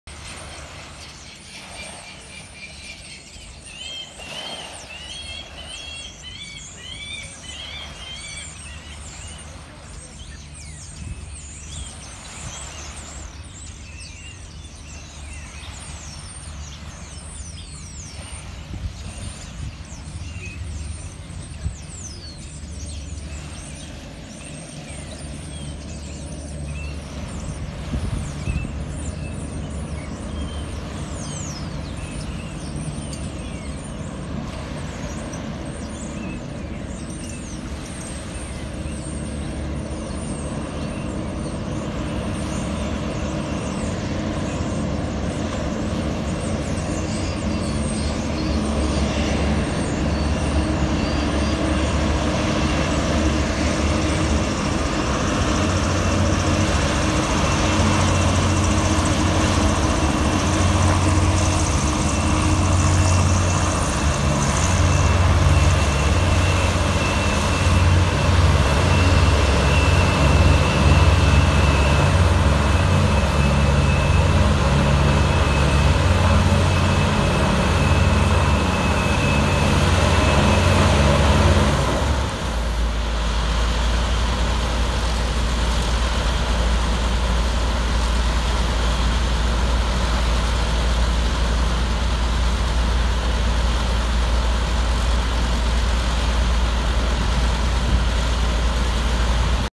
Scenes from Southsea
Hovertravel’s hovercraft landing at Southsea from Ryde